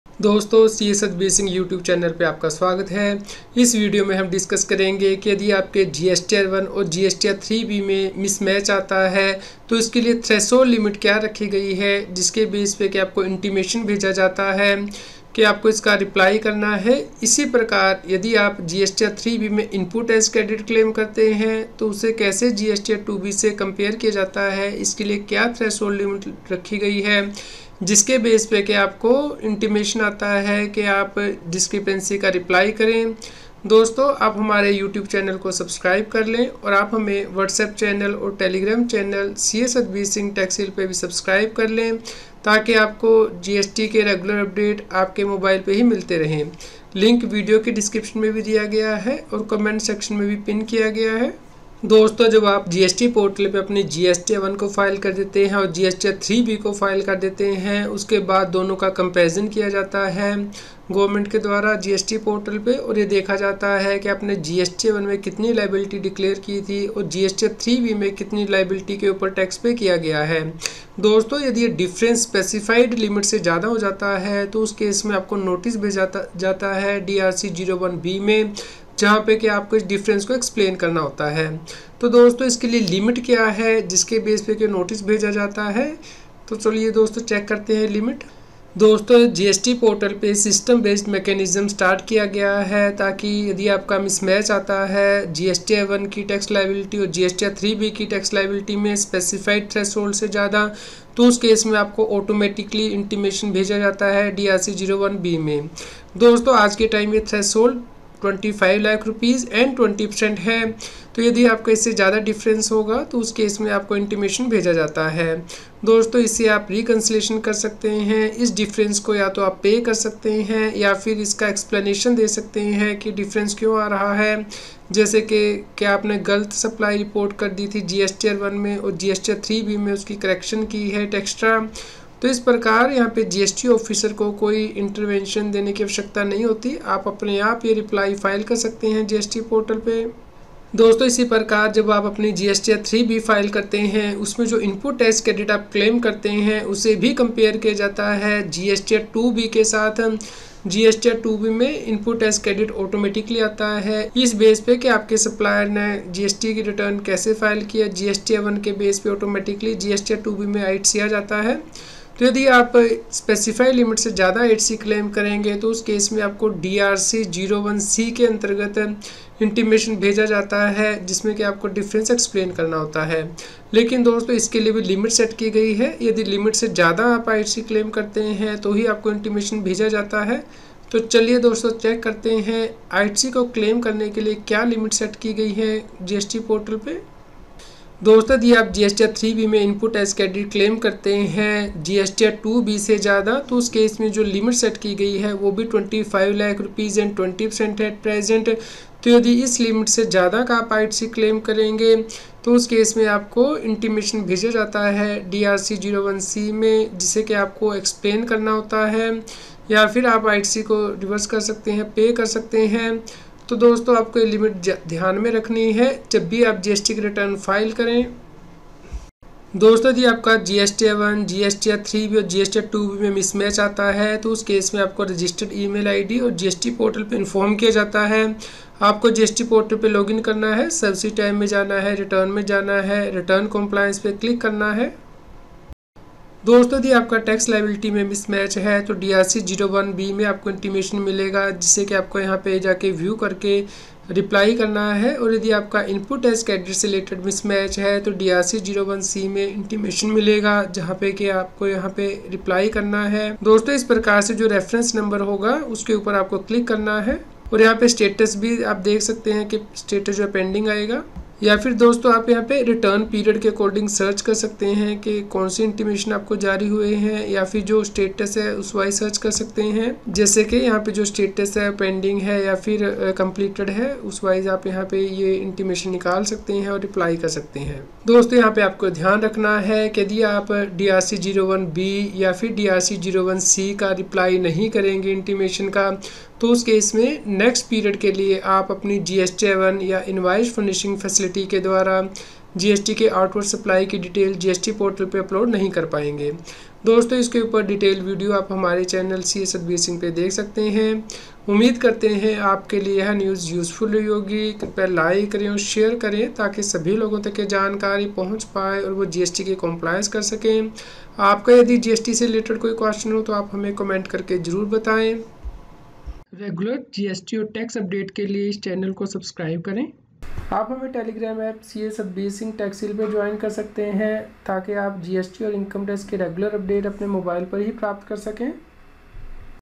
GST NOTICE NEW LIMIT FOR MISMATCH OF GSTR 1 GSTR 3B & GSTR 2B I DRC 01B I DRC 01C GST RETURN FILER
NEW LIMIT FOR GST NOTICE FOR MISMATCH OF GSTR 1 GSTR 3B & GSTR 2B I DRC 01B DRC 01C GST RETURN FILER